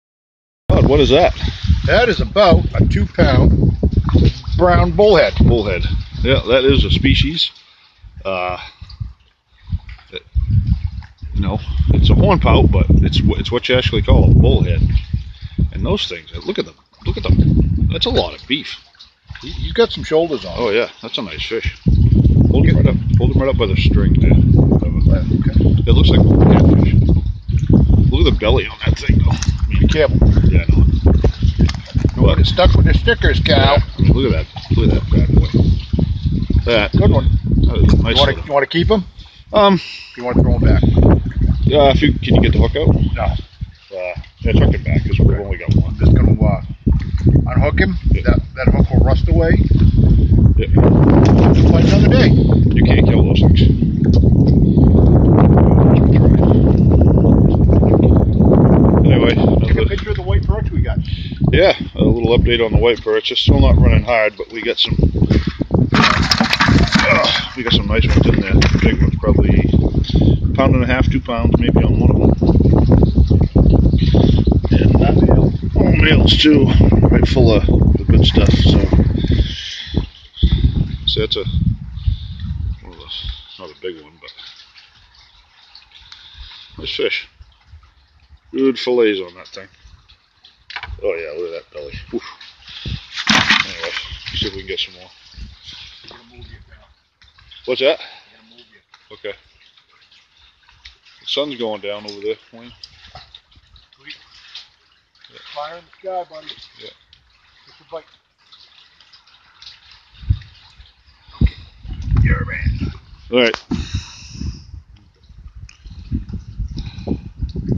A BULLHEAD AND WHITE PERCH UPDATE
Wayne caught a nice bullhead which is basically a hornpout. The white perch are starting to get active and we caught a few nice one's. It was a good night fishing.

"Making a Longbow from Ash, Part 5 (Staining & Painting)"